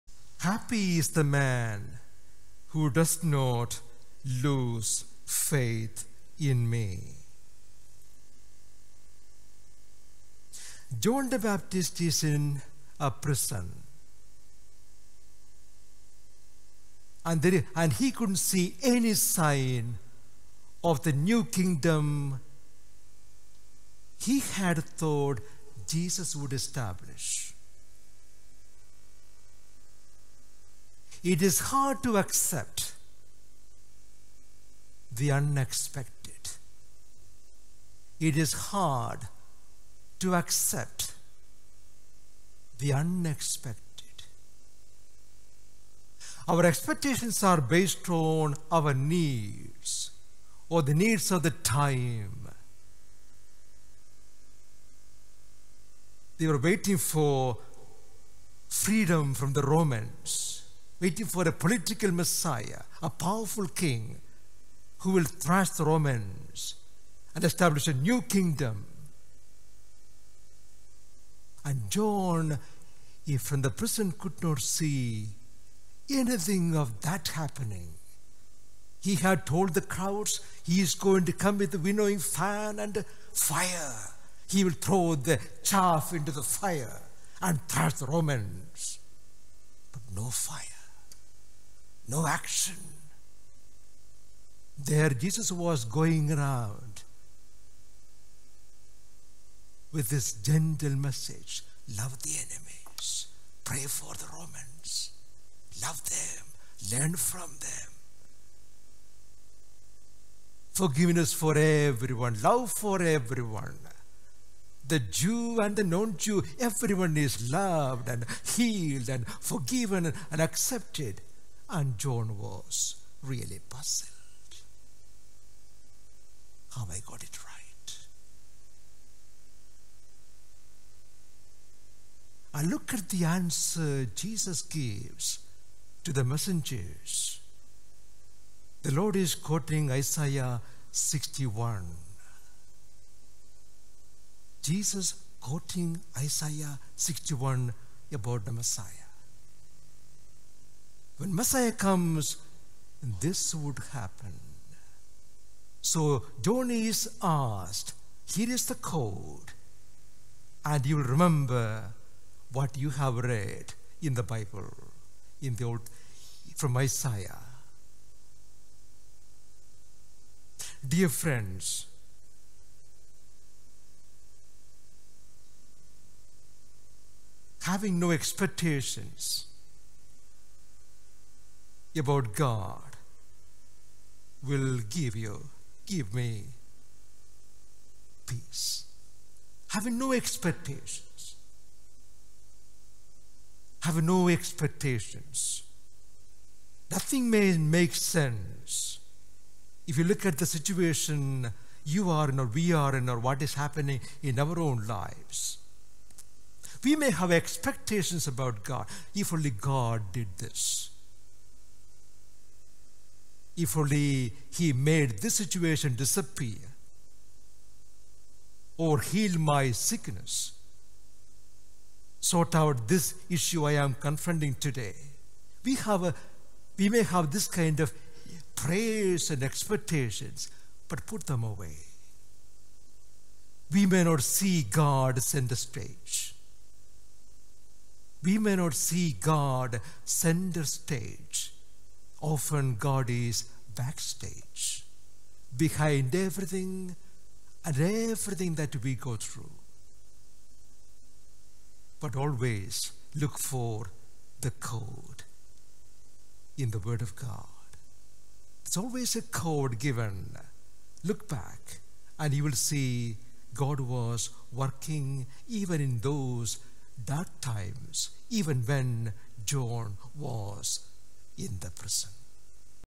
Homily 16th December 2020 LK 7:18-23 (Advent Week 3 Wednesday)
God who upends our expectations and exceeds our expectations! Don't have expectations about God who is never centre stage but always back stage. Hold on to the Word of God and to the 'code'!